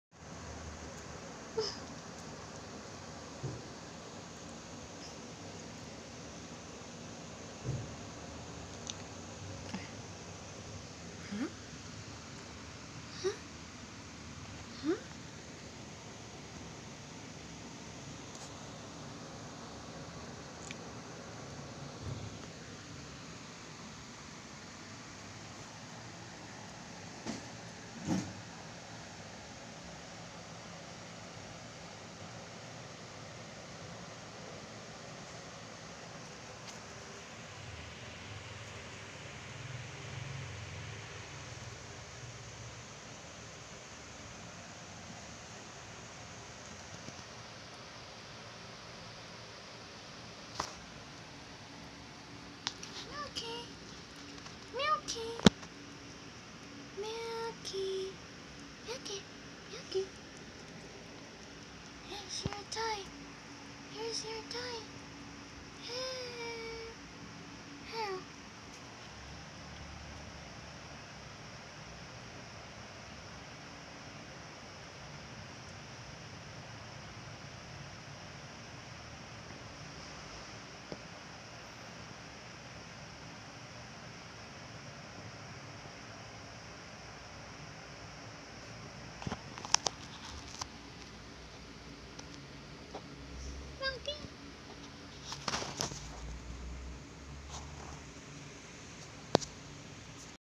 My new baby, Milky!
Milky is a 1-month dalmador (dalamatian-labrador)
I got him from a side walk near gaisano. Sobrang naawa ako sakanya nakupo sya at nakayuko habang natutulog sa ilalim ng araaw! Nadurog ang puso ko pero nakyutan ako sknya kasi muka syang polar bear. Hehe

Also on instagram: lanilaneocbina